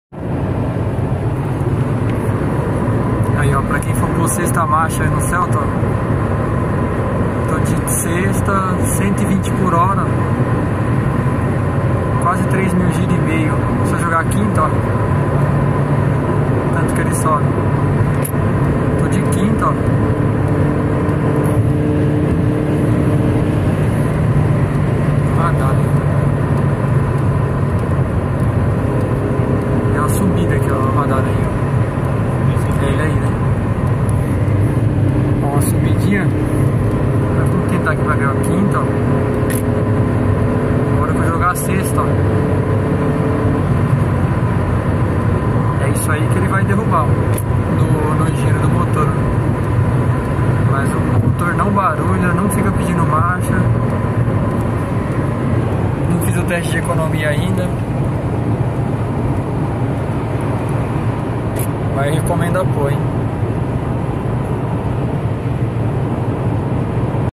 6 MARCHAS NO CELTA, FIZ O TESTE DE ROTAÇÃO NA RODOVIA.
Fiz o teste de quantos giros a rotação do motor cai na rodovia usando quinta e sexta Marcha. Será que compensa trocar o câmbio de 5 para 6 marchas?